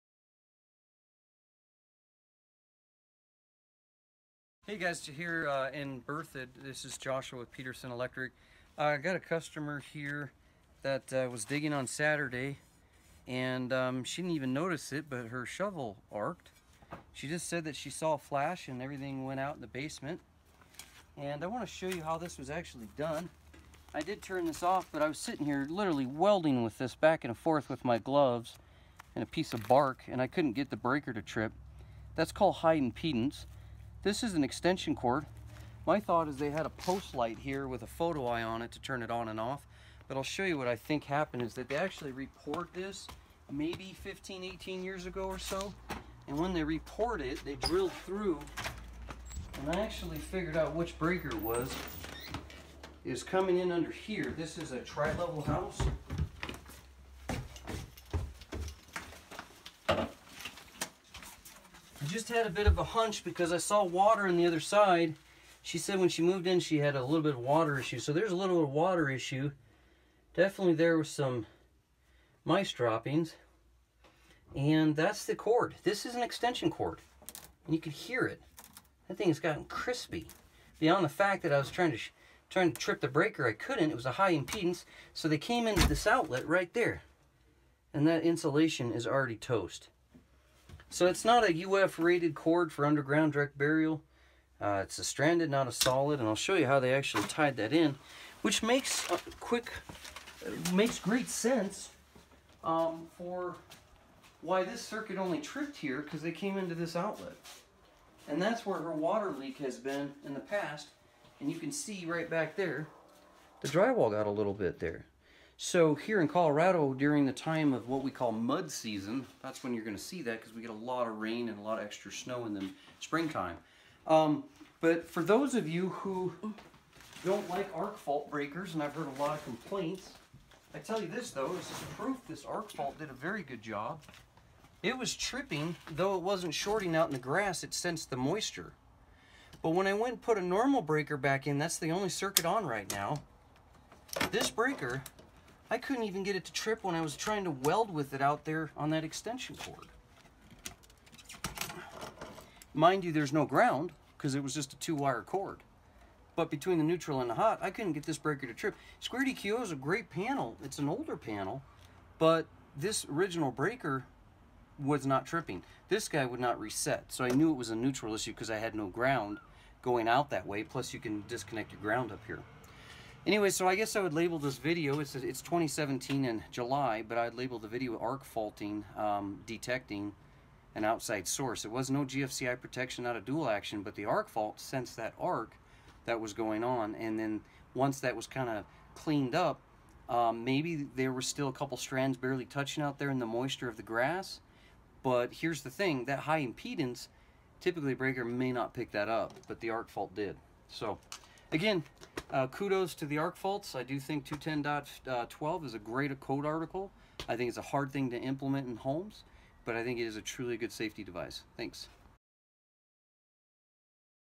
Shoveling In Yard, Extension Cord In Ground, Fire Hazard & Power Lost In House
Homeowner lost power to parts of the house, due to shoveling in the yard and damaging a wire that was installed incorrectly. The wire was installed barely in the ground. This could have been a very dangerous situation.